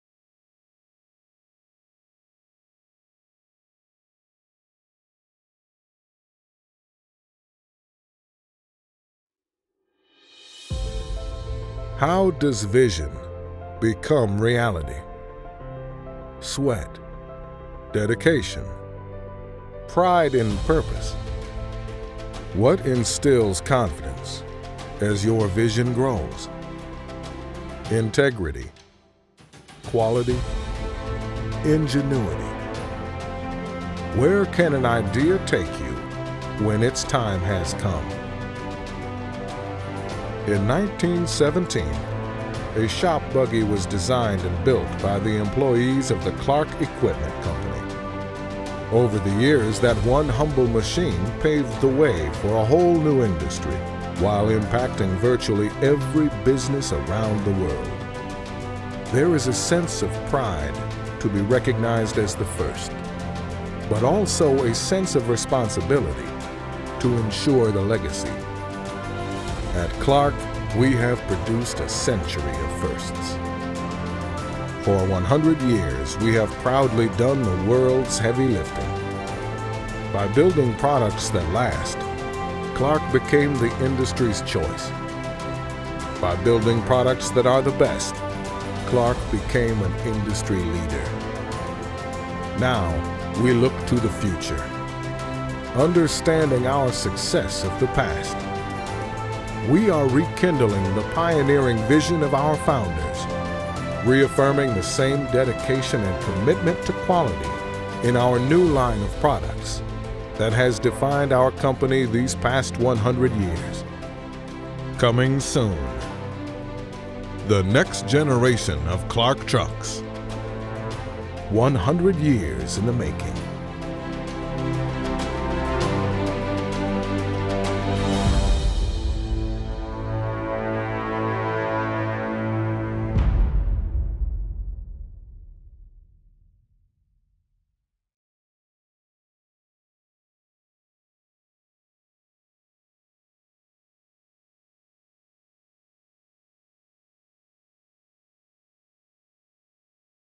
CLARK Centennial Promotional Video
After a Century of innovation from CLARK, the stage is set for The Next Generation Lift Truck.